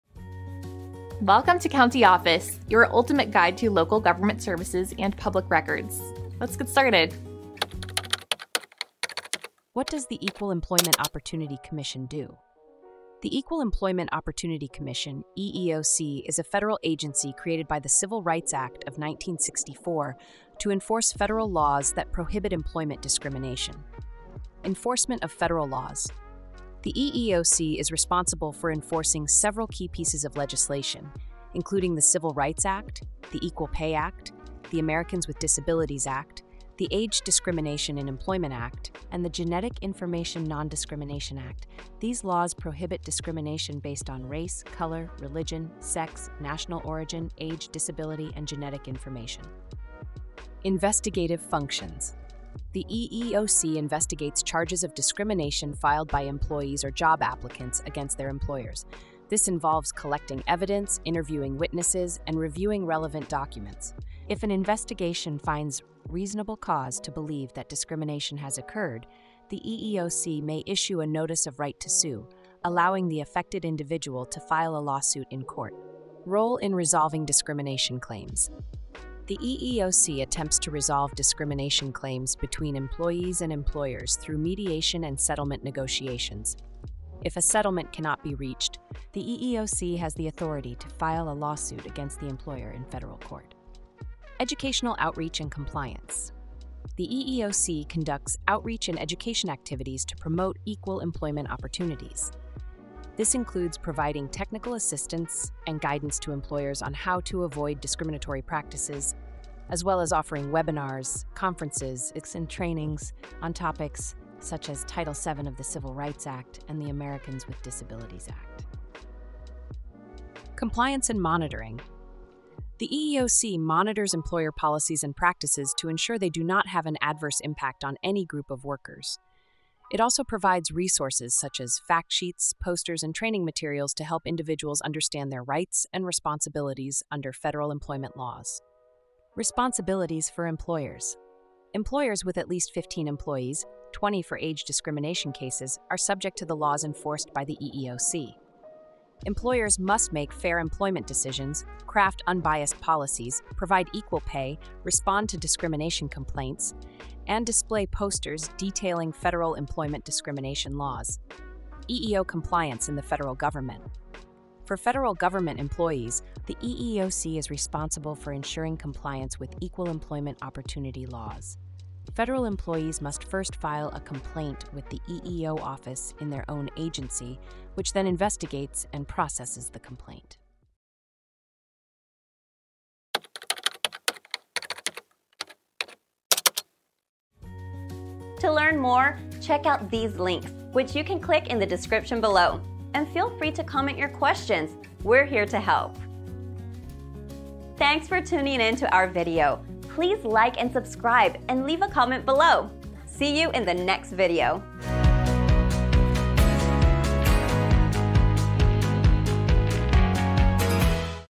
What Does The Equal Employment Opportunity Commission Do? - CountyOffice.org
What Does The Equal Employment Opportunity Commission Do? Have you ever considered the role of the Equal Employment Opportunity Commission (EEOC) in protecting your rights in the workplace? In this informative video, we will delve into the essential functions of the EEOC, a federal agency established to enforce laws that prohibit employment discrimination. We’ll explore the key pieces of legislation that the EEOC enforces, including the Civil Rights Act, the Equal Pay Act, and the Americans with Disabilities Act. Understanding the investigative functions of the EEOC is crucial, as it plays a vital role in addressing allegations of discrimination by collecting evidence and interviewing witnesses.

We will also discuss how the EEOC works to resolve discrimination claims through mediation and settlement negotiations, as well as its authority to file lawsuits when necessary. Additionally, we’ll highlight the educational outreach initiatives provided by the EEOC, which aim to inform both employers and employees about their rights and responsibilities under federal employment laws.

Whether you are an employee seeking to understand your rights or an employer looking to ensure compliance, this video is packed with valuable insights.